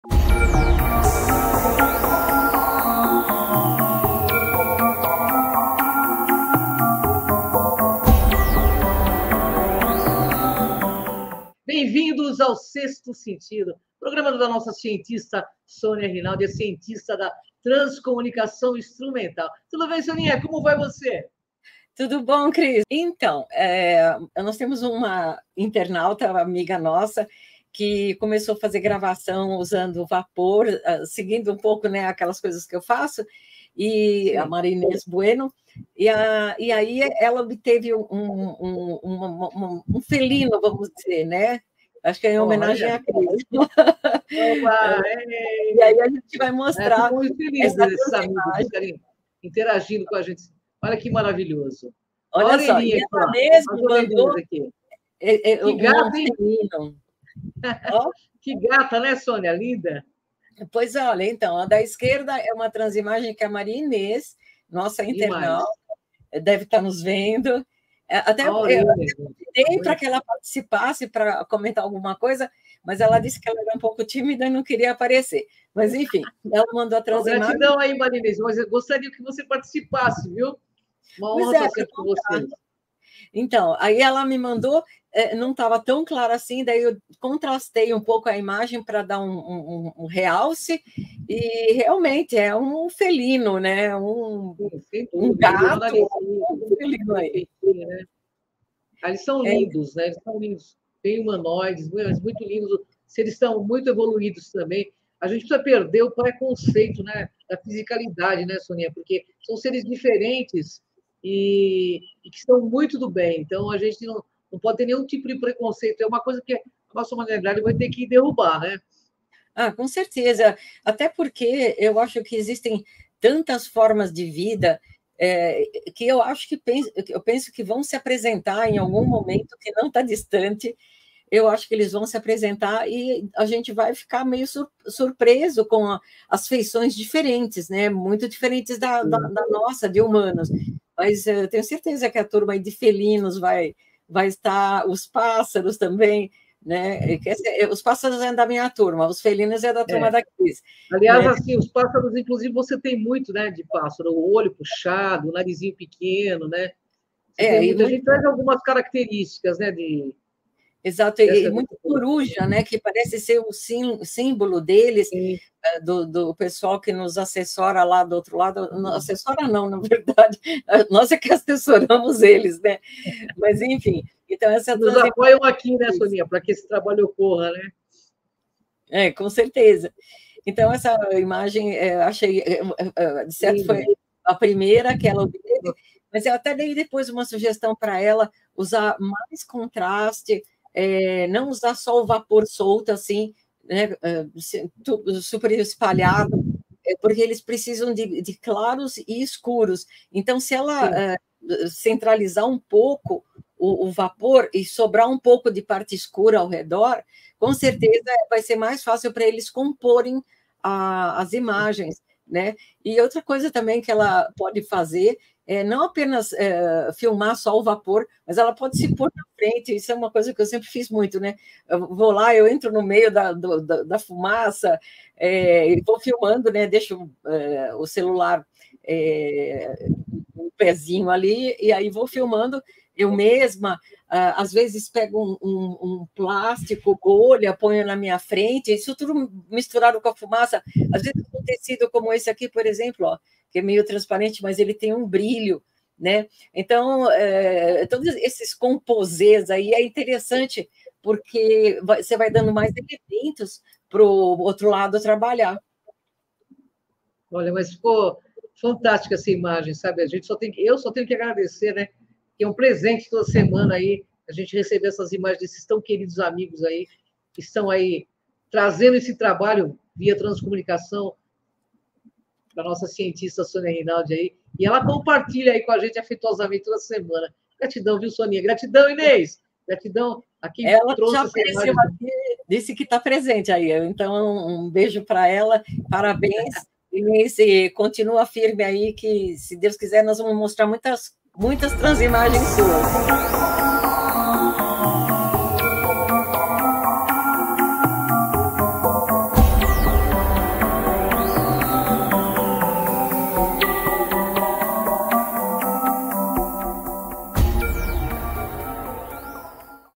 Entre Dois Mundos: Transimagem de um Felino Espiritual Revelada por Sonia Rinaldi e Maria Inês.
Neste vídeo sepamos esse trecho da Live do canal @KROONTVEvolution realizado pela Cris D´Paschoal, todas as quintas feira às 18:30h, onde Sonia Rinaldi compartilha transimagem de uma escrita e admiradora de seu trabalho que está levando  a sério a pesquisa em Transimagem chamada Maria Ines, a mesma obteve uma evidencia incrivel de um ser espiritual com caracteristica felina, veja e compartilhem.